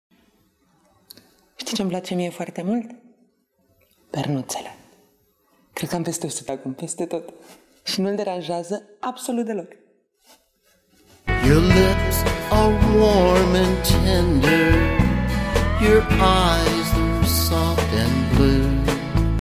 Volkswagen Polo - Long Distance Love: My cushions
A couple discovers the benefits of long distance relationships.
Thanks to the new Polo which only uses 3.4 liters for every hundred kilometers.